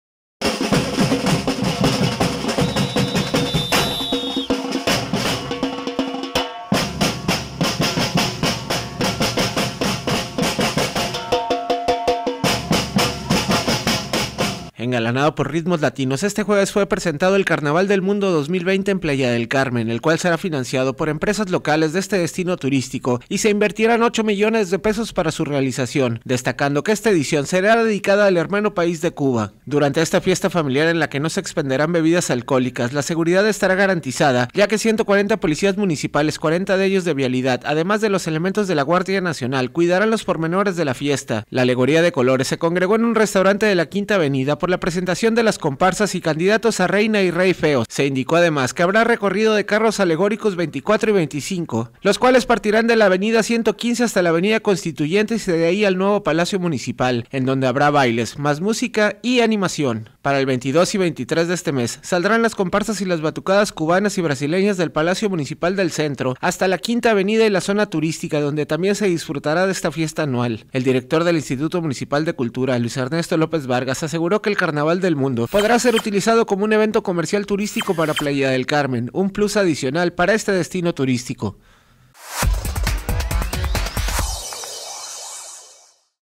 PRESENTAN EL “CARNAVAL DEL MUNDO 2020” DE PLAYA DEL CARMEN
Entérate Engalanado por ritmos latinos, este jueves fue presentado el “Carnaval del mundo 2020” en Playa del Carmen, el cual será financiado por empresas locales de este destino turístico y se invertirán ocho millones de pesos para su realización, destacando que esta edición será dedicada al hermano país caribeño, Cuba.